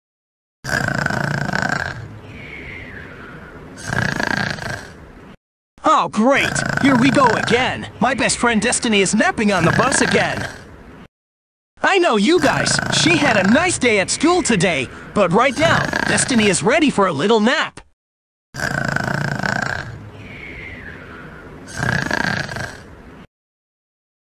My best friend princess Rosario is napping on the bus once again 😊😊😊😊😊😊💙💙💙💙💙💙💙
Apparently while we’re ready to go home